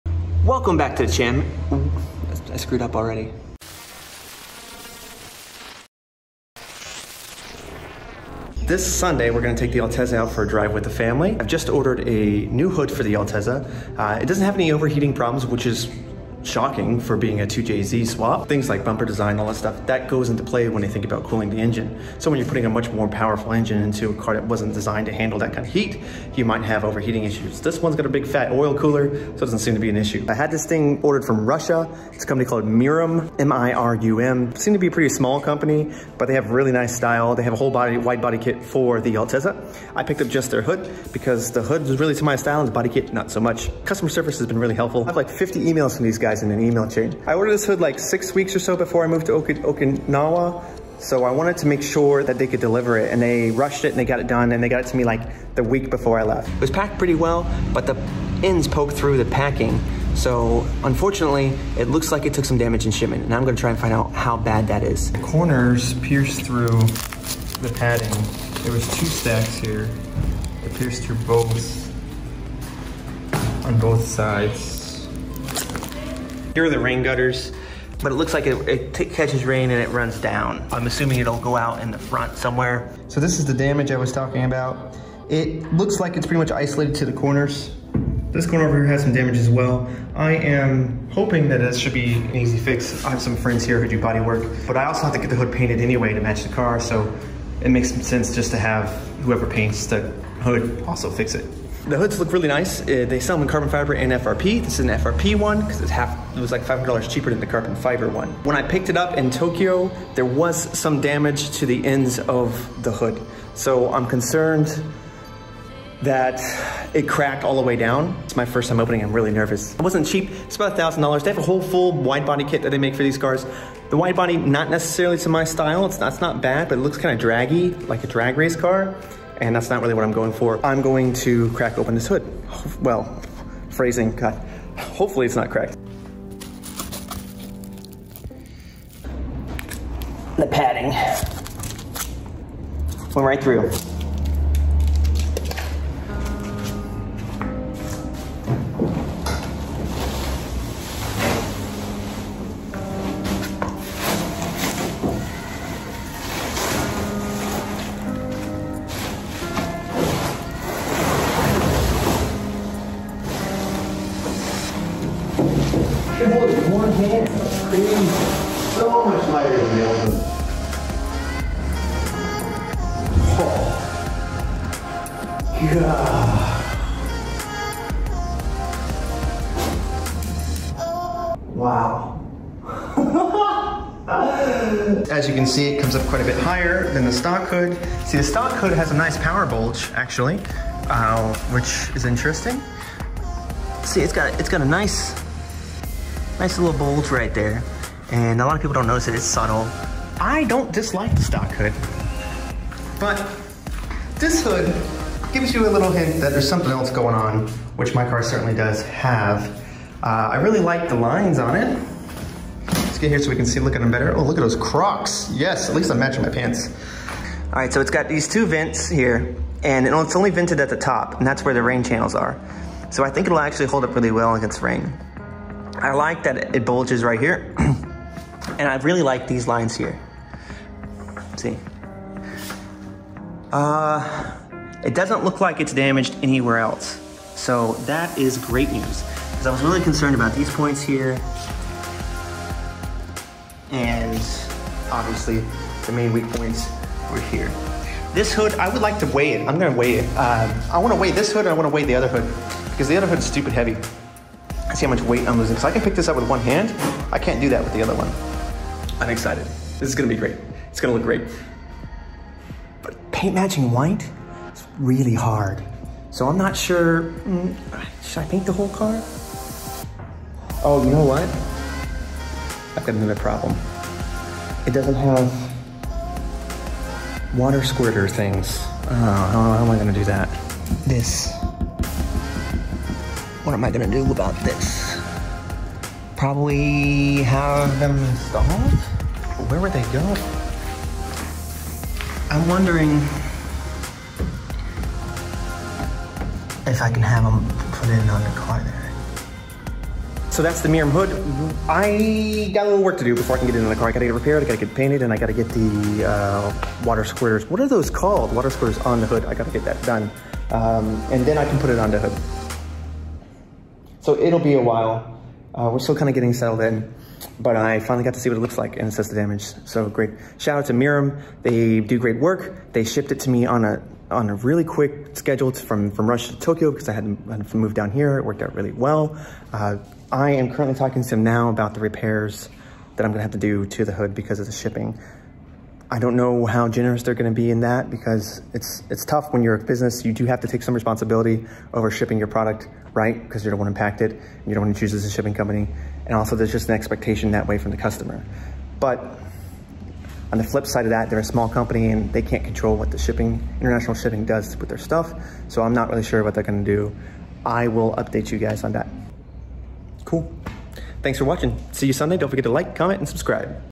$1,000 Hood Damaged in Shipping! | 500 HP Altezza Update
Sorry for the absence guys, I've been moving house. My personal items have finally arrived which means its time to look at the new hood for the Altezza!

Sadly, it was a victim of shipping and took a bit of damage. However, it's largely in very good shape and I'm excited to get it cleaned up and on the car!

Aftermarket body kits can so easily be tacky or distasteful, so huge shout out to Mirum Bodykits for making quality products! I can't wait to see what else they do in the future.

00:00 Intro
00:09 Why a new hood?
00:43 Mirum Bodykits impressions
01:00 The damage
02:32 Hood reveal
03:05 Comparison to stock hood
05:12 Conclusion